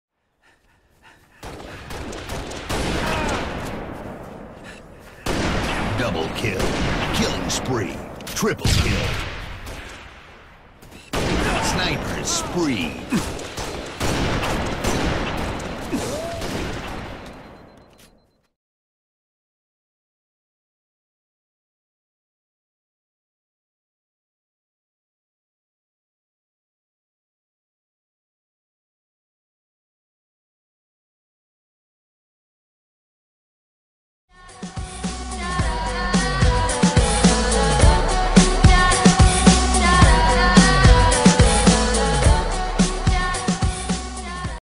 Epic Quick Scope for the NOverkill!
So close to awesomeness! Then screwed for the Tac..
I added some new editing to my daily clips. Let me know what you think :)